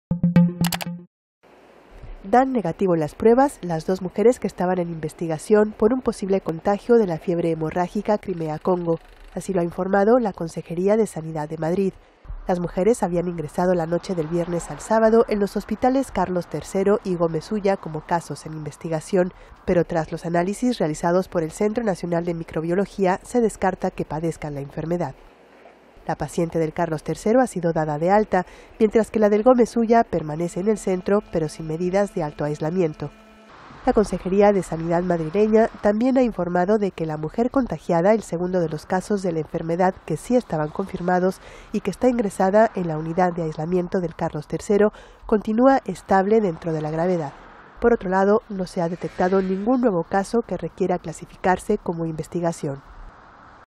Las dos sospechosas de tener fiebre hemorrágica dan negativo en las pruebas
Madrid/Valladolid, 4 sep (EFE).- La Consejería de Sanidad de Madrid ha informado de que la situación clínica de la paciente ingresada en la Unidad de Aislamiento del Hospital La Paz-Carlos III continúa "estable dentro de la gravedad", y que las dos pacientes ingresadas con sospecha de tener el virus, una de ellas que había viajado a Ávila, han dado negativo.

Se descarta que las dos mujeres, que ingresaron la noche del viernes al sábado en los hospitales Carlos III y Gómez Ulla como casos en investigación, padezcan la fiebre hemorrágica Crimea-Congo, tras los análisis realizados por el Centro Nacional de Microbiología que han resultado negativos.

La paciente de La Paz-Carlos III ha sido dada de alta hospitalaria, mientras que la mujer ingresada en el Gómez Ulla permanece en el centro, pero sin medidas de alto aislamiento, y la necesidad o no de mantenerla ingresada será determinada por los facultativos, ha agrega Sanidad.

La mujer que permanece en el Gómez Ulla explicó en un primer momento a los sanitarios del madrileño hospital Ramón y Cajal que había paseado por una zona de Ávila y que una garrapata había picado en una pierna a su marido, quien presentó luego síntomas de fiebre pero que ahora se encuentra bien.

Ella fue al centro sanitario al notar fiebre y algún otro síntoma compatible con la enfermedad y se la trasladó al Gómez Ulla.

Esta mujer visitó un pueblo de Ávila junto a su marido, en el entorno de Solosancho, en una comarca abulense distinta de la que surgió el único caso de víctima mortal por fiebre hemorrágica Crimea-Congo en Europa Occidental.

No se ha detectado ningún nuevo caso que requiera clasificarse como en investigación, según han precisado desde la Consejería de Sanidad madrileña.

Ese departamento se mantiene en contacto permanente con el Ministerio de Sanidad, Asuntos Sociales e Igualdad para coordinar las medidas que fueran necesarias.

La Comunidad de Madrid mantiene a disposición de los ciudadanos información en el Portal Salud y en la web sobre la Enfermedad Hemorrágica Crimea-Congo.